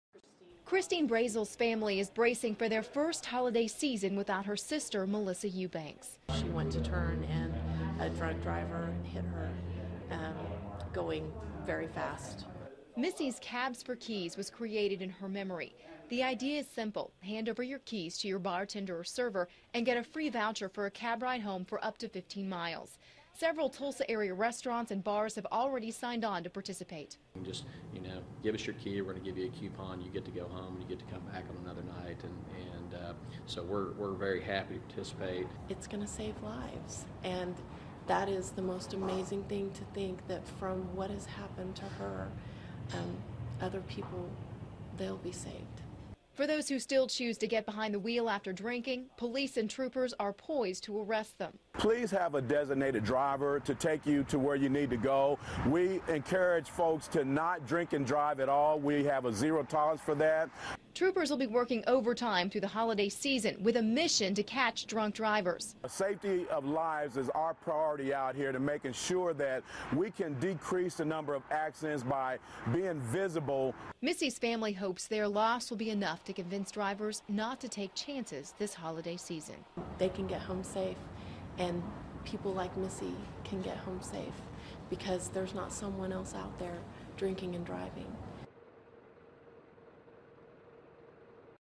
Free cab rides for intoxicated drivers
Several Tulsa area bars and restaurants have agreed to participate in a program to offer cab rides for those who choose to drink over the holiday weekend.